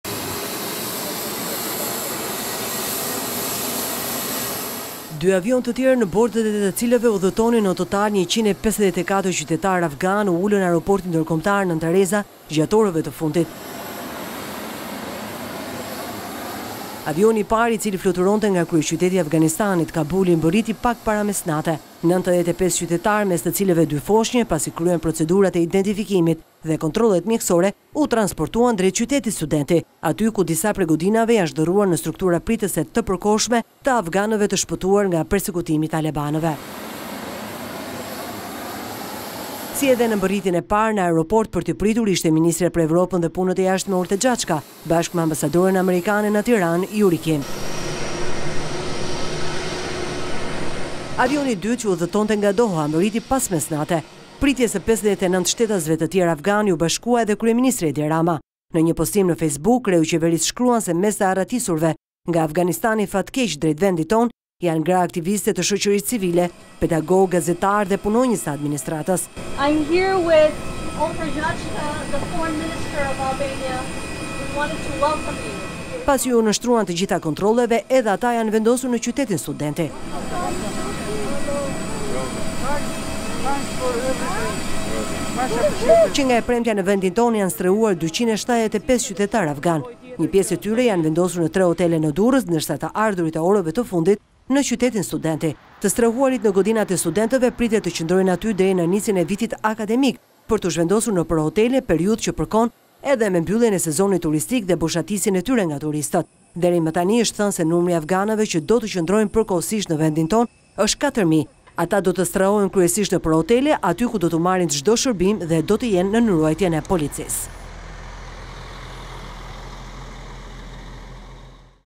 Rama pret vetë avionin e radhës me afganë në Rinas, 154 persona sistemohen në "Qytetin Studenti" -
Kryeministri Edi Rama bëri bashkë këtë të premte socialistë të vjetër e të rinj, emra që do të bëhen deputetë për herë të parë dhe ata që nuk do të ulen më në karrigen e Kuvendit.Në takimin në “Pallatin e Brigadave”, Rama nënvizoi se Partia Socialiste, përtej prurjeve dhe ikjeve, do të mbetet një familje e bashkuar. Ai veçoi emrin e ish-kryeparlamentarit Gramoz Ruçi, që u dorëhoq, duke deklaruar se ai do të vijojë të qëndrojë në krahun e tij të djathtë.“Ne këtu jemi dhe do të jemi një skuadër, që nga Gramozi, i ciliu do të vijojë në krahun tim të djahtë, edhe pse tani I vetëçliruar nga çdo funksion publik. Askujt të mos i vijë keq, por askush nuk e zë do të atë vend. Deri tek deputeti më i ri në këtë skuadër, që për herë të parë do të kalojë pragun e Kuvendit të Shqipërisë. Ne jemi një skuadër dhe ndryshimet e roleve herë pas here janë vetëm në funksion të skuadrës. Nuk do të jetë asnjëherë e tepërt që të falënderoj deputetët dhe deputetet që e përfunduan punën e tyre të mandatit të dytë, duke dhënë kontributin në Kuvend, në periudhën e vështirësive më të jashtëzakonshme për një udhëheqje politike. Ata që nuk do të jenë më në Kuvend, do të kenë mundsinë të përfshihen në punët e mëdha të partisë. Do të vazhdojmë punën për zgjedhjet vendore te 2023 dhe zgjedhjet parlamentare të 2025. Çfarë do të mbjellim çdo ditë nga sot e deri atëherë, do të korrim atë që populli do të ndajë të njomët nga të thatët.”